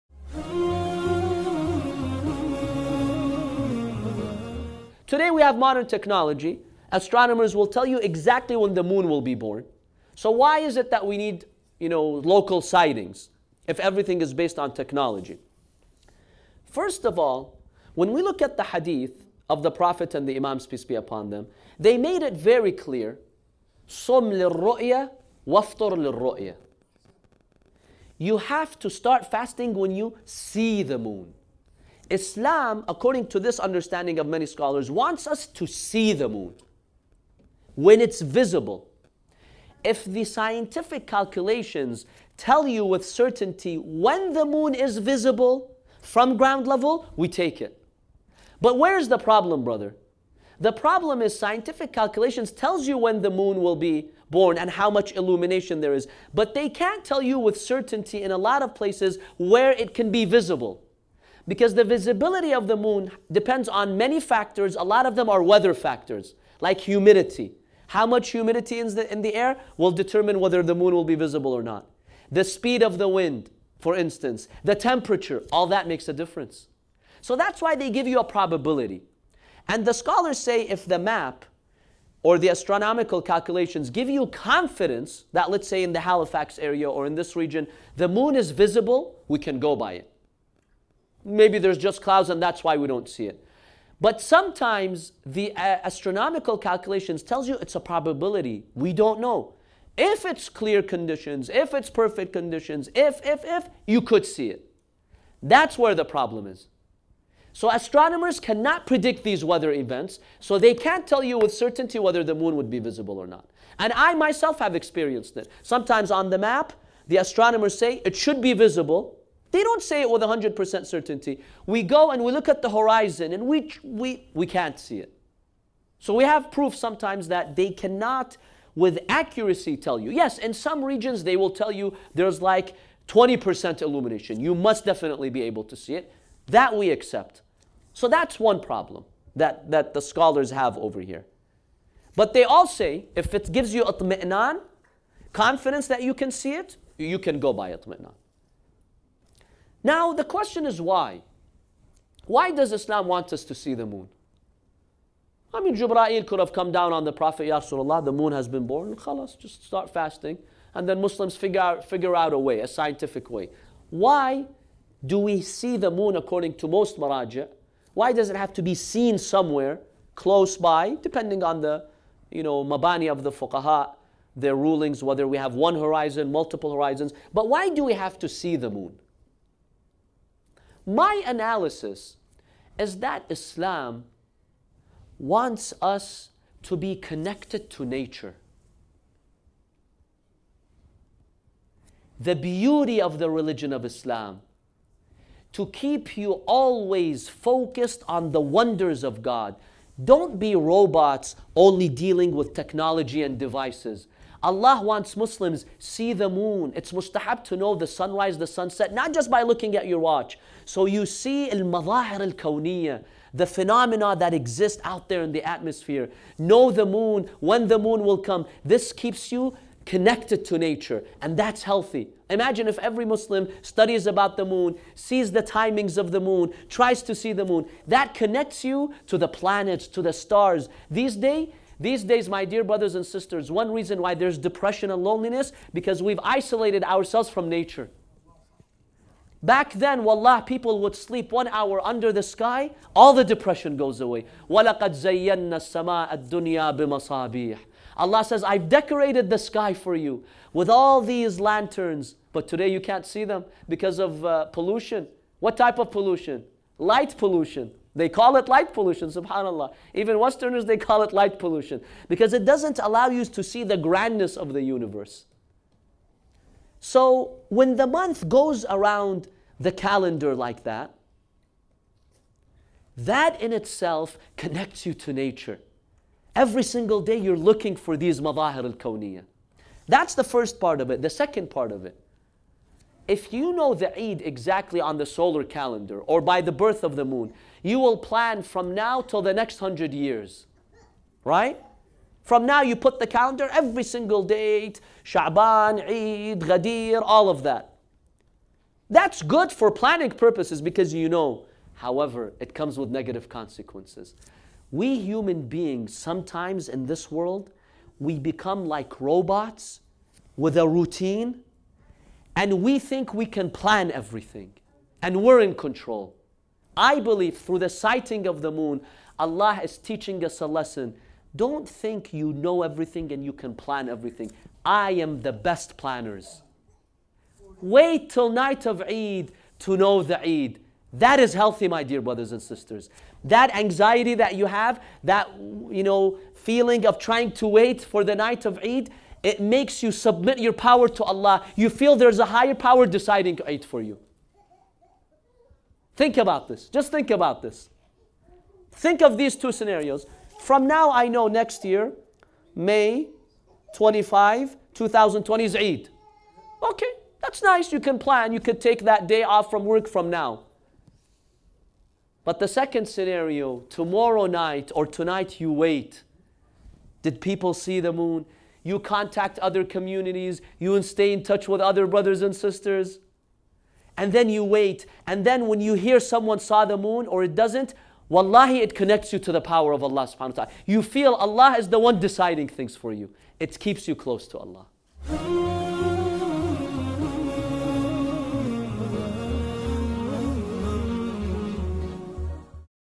Why do we need local Moon Sightings when we can use Modern Technology? - Sayed Baqer Al-Qazwini
Al-Rasoul Islamic Society || Halifax - Canada